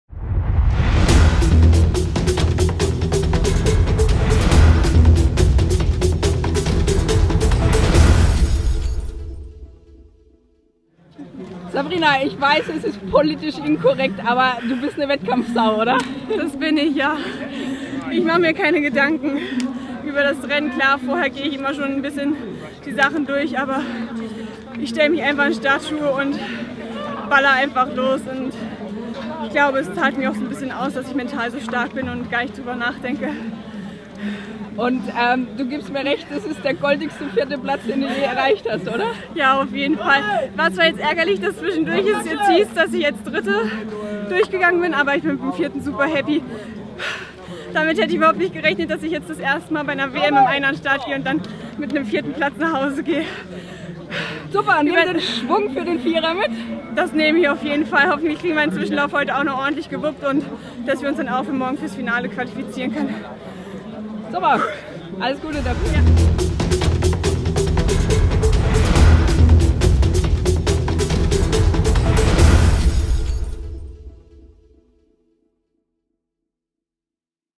Interview mit Sabrina Hering
Zum ersten Mal bei einer WM im Einerkajak über die olympischen 500 Meter unterwegs und schon so nah an der Medaille. Ein wahnsinns Rennen von Sabrina Hering und wie immer ein super-sympathisches Interview.